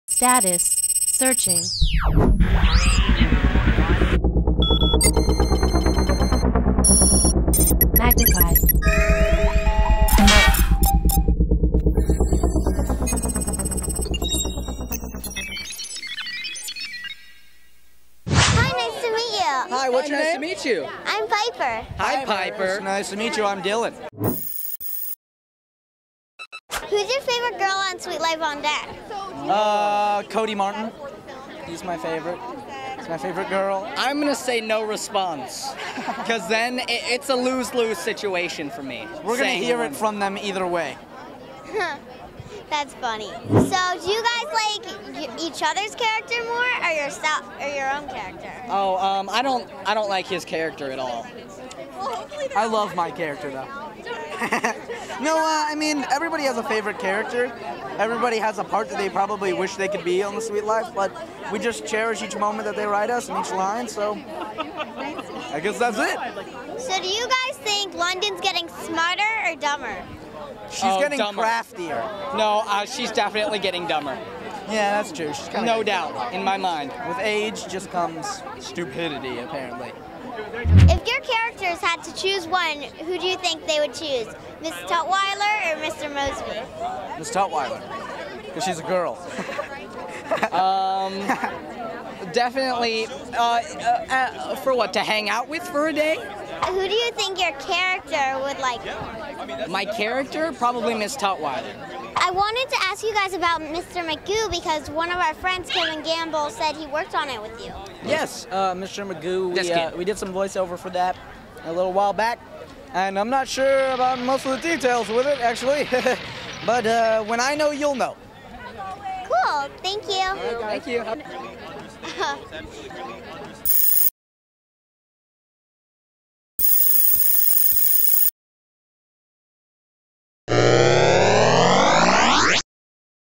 Dylan and Cole Sprouse Suite Life Interview Hollywood Entertainment Pre-Teen Reporter Piper Reese!
Dylan and Cole Sprouse Suite Life Interview with Planet's Youngest Entertainment Reporter Princess of the Press Piper Reese! (PQP #017)
OMG!  Celebrity super-tween interviewer, entertainment reporter (and planet's youngest podcaster) Piper Reese pulls an interview out of the Piper-vault!  It's Piper on the red carpet with Dylan and Cole Sprouse!  Yeah baby!  It's PiPeRiFiC Piper's QUICK Picks episode with the super-funny dynamic duo Cole and Dylan!  (No Danimals yogurt has been harmed in the editing of this video.)